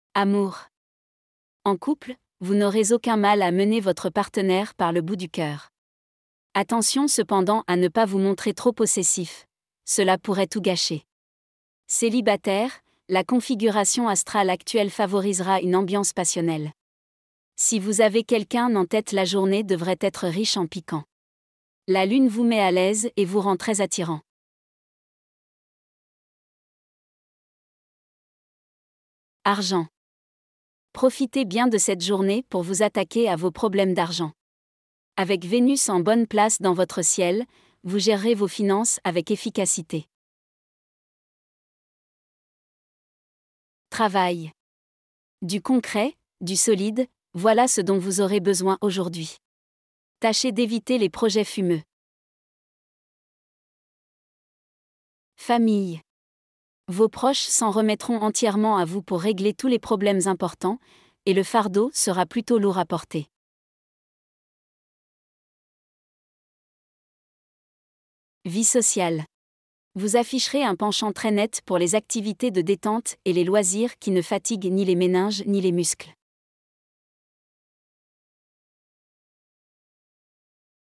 Capricorne, votre horoscope du 19/12/2020
Amour : En couple, vous n'aurez aucun mal à mener votre partenaire par le bout du coeur. Attention cependant à ne pas vous montrer trop possessif : cela pourrait tout gâcher. Célibataire, la configuration astrale actuelle favorisera une ambiance passionnelle. Si vous avez quelqu'un en tête la journée devrait être riche en piquant. La Lune vous met à l'aise et vous rend très attirant.
Argent : Profitez bien de cette journée pour vous attaquer à vos problèmes d'argent. Avec Vénus en bonne place dans votre Ciel, vous gérerez vos finances avec efficacité.
Santé : Votre sommeil risque d'être perturbé par le mauvais aspect de Neptune. En revanche, vous ferez de nombreux rêves intéressants.
Travail : Du concret, du solide, voilà ce dont vous aurez besoin aujourd'hui. Tâchez d'éviter les projets fumeux.
Famille : Vos proches s'en remettront entièrement à vous pour régler tous les problèmes importants, et le fardeau sera plutôt lourd à porter.
Vie_sociale : Vous afficherez un penchant très net pour les activités de détente et les loisirs qui ne fatiguent ni les méninges ni les muscles.